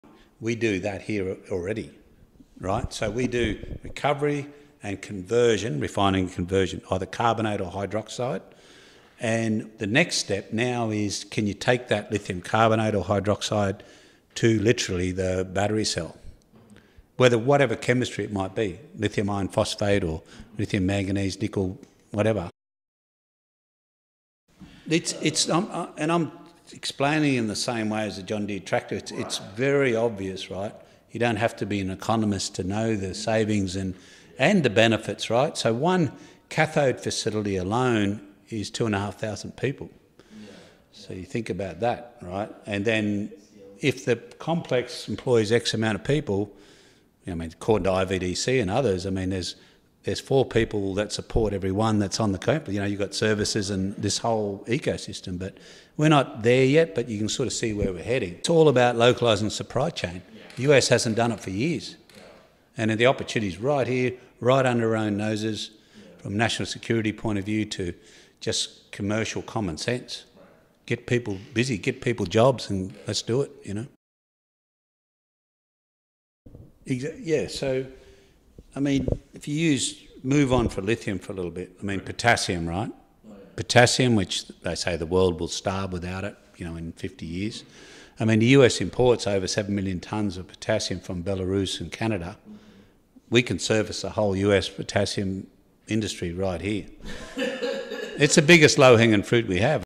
Rebuilding America: Lithium & Potassium Supply Chains
Revitalizing domestic supply chains for essential resources like lithium and potassium is key. Learn how this strategy fosters economic growth, job creation, and national security. This is a common sense approach.